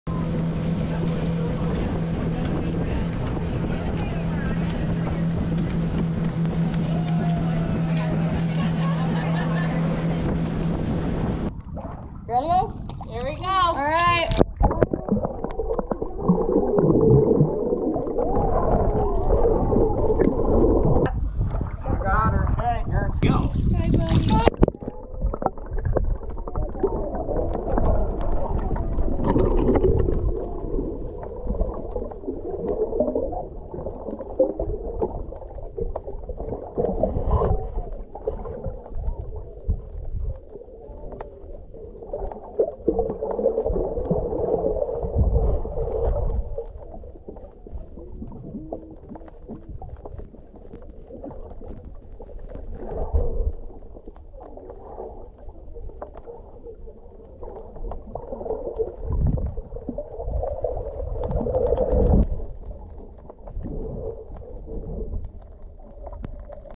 Sea turtles released into the Gulf of Mexico as part of conservation program
Two sea turtles were released back into the ocean by a conservation organisation in Marathon, Florida, video shared online showed. This footage, filmed by the Turtle Hospital, shows two juvenile loggerhead sea turtles, Ethel and Lucy, being transported into the Florida Keys, where they are then released. BE ON IT WITH THE LATEST NEWS AND LIFESTYLE VIDEOS